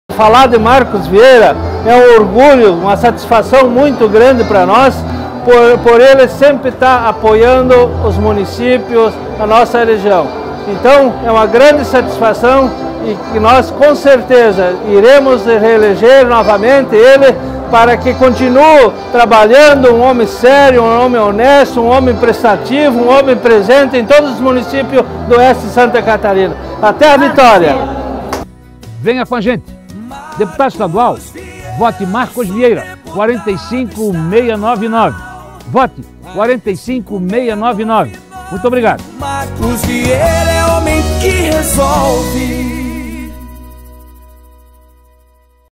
Apoio Amarildo Damo - Presidente PSDB SERRA ALTA
Serra Alta está com o 45699
O presidente do PSDB, Amarildo Damo, do município de Serra Alta apoia a campanha de Marcos Vieira para Deputado Estadual SC.
Marcos Vieira 45699